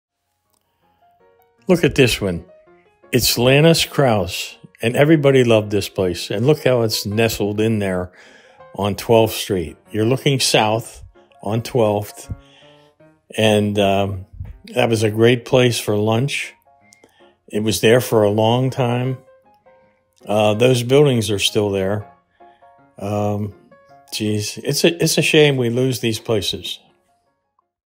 Lanos Kraus in Downtown Wheeling WV
Please Click to View the Story. This is a series of videos of historic insights into Wheeling WV history.